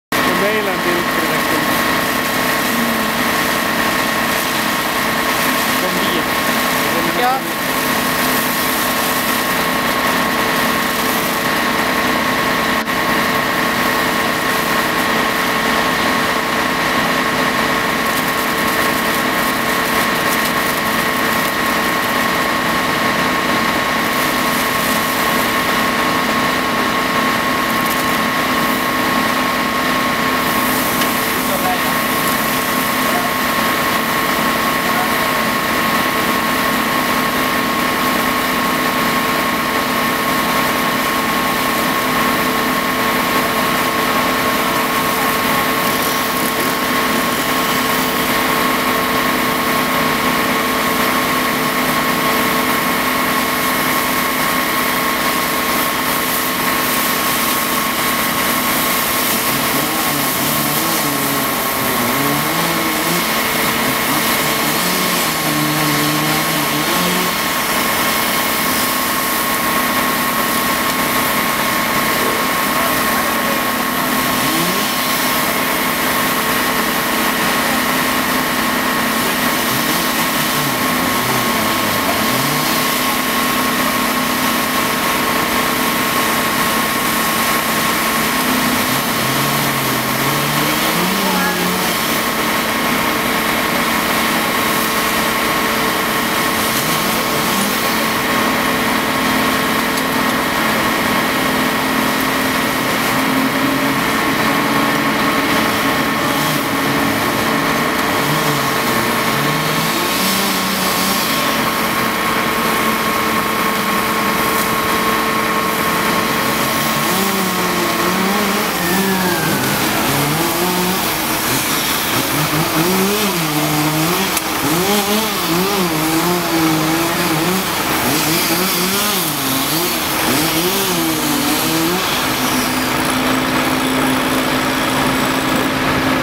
brand1
Egnahemshus i korsningen Centralgatan-Smedsbyvägen brinner.